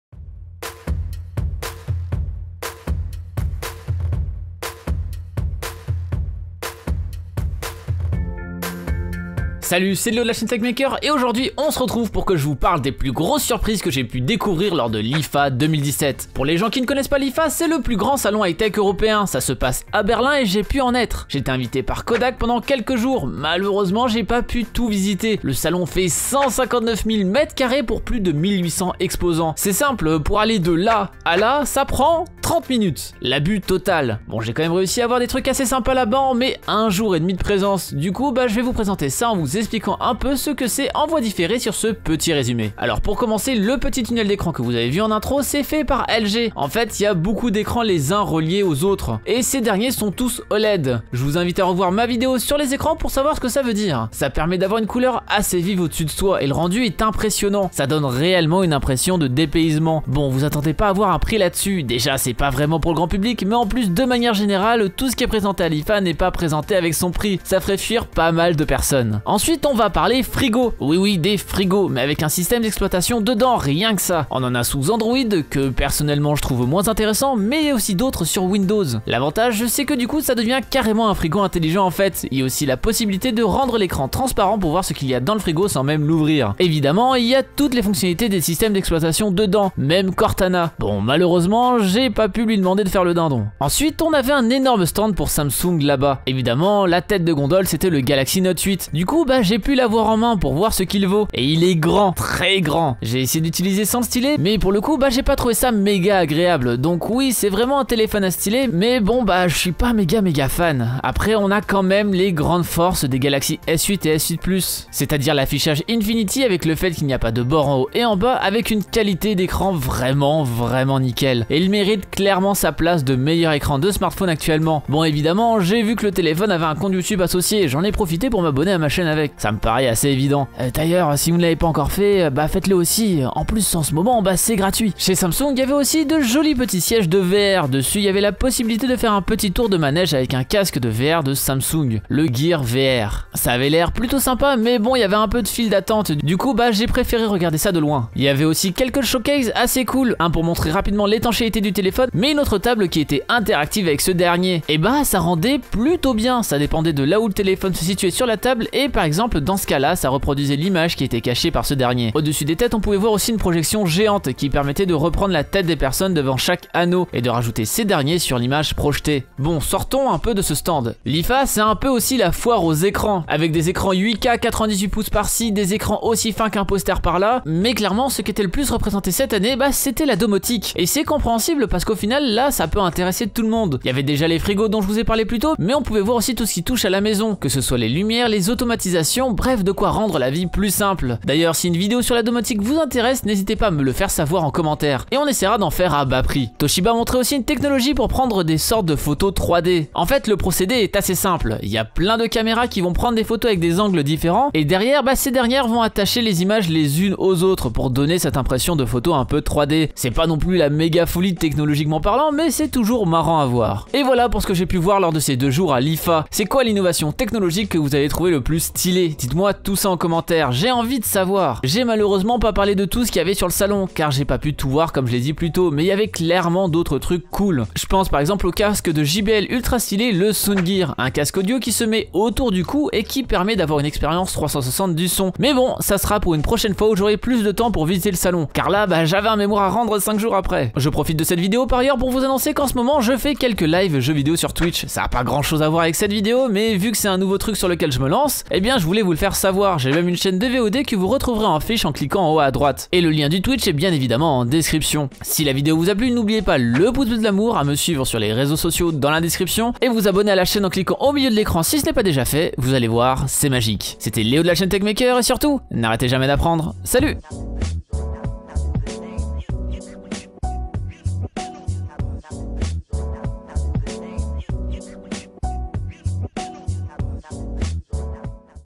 WINDOWS 10 SUR UN FRIGO ?!
Salut c'est Léo de la chaîne Techmaker et aujourd'hui je vous montre les plus grandes innovations que j'ai pu voir à l'IFA, comme un frigo LG sous Windows ou bien même le nouveau Samsung Galaxy Note 8 !

Et n'hésitez pas non plus à commenter, je répond très souvent :)

★ Essayer GRATUITEMENT Amazon Prime pendant 6 mois
★ Les meilleurs bons plans sur Amazon
★ Les meilleurs bons plans sur Banggood